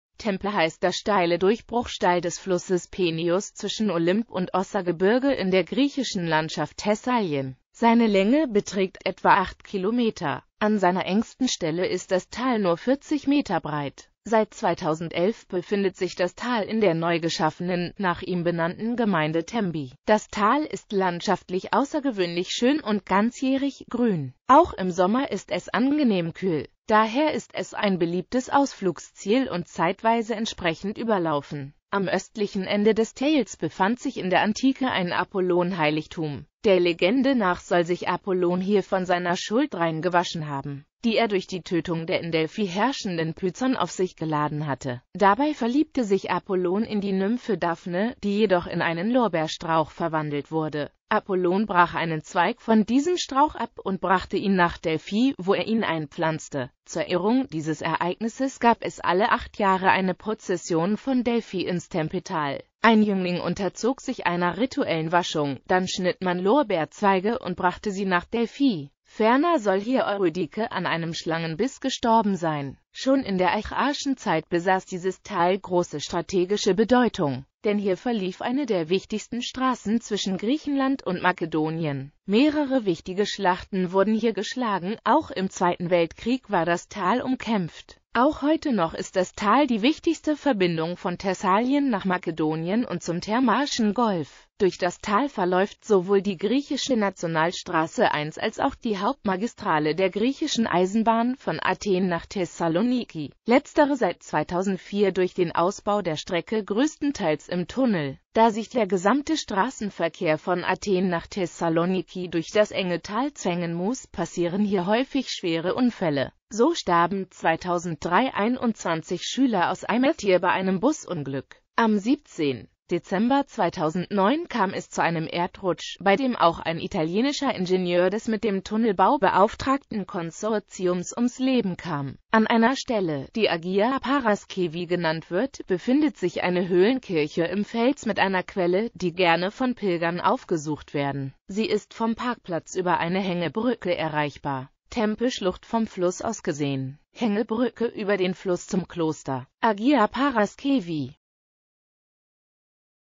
Tempe (Griechenland)
Tempe (griechisch Τέμπη, altgr.Tempē, neugr.Tembi) heißt das steile Durchbruchstal des Flusses Peneios zwischen Olymp- und Ossa-Gebirge in der griechischen Landschaft Thessalien.Seine Länge beträgt etwa 8 km.